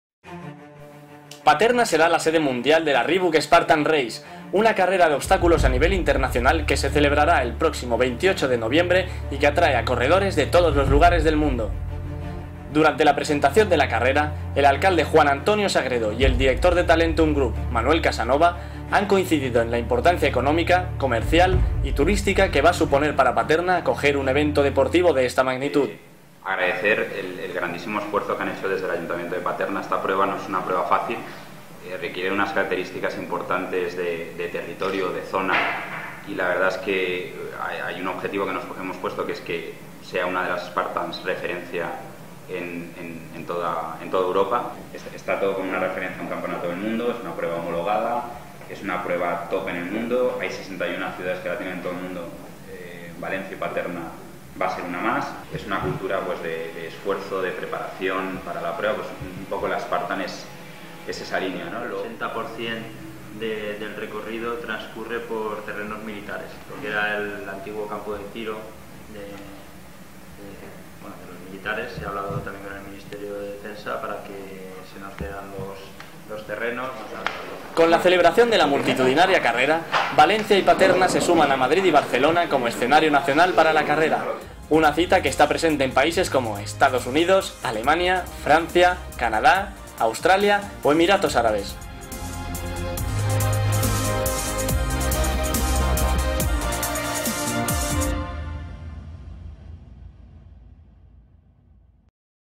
La Reebok Spartan Race se celebrará en Paterna el 28 de noviembre
Rueda de prensa del alcalde Juan Antonio Sagredo junto a Manuel Casanova de Talentum Group para la presentación de la Reebok Spartan Race.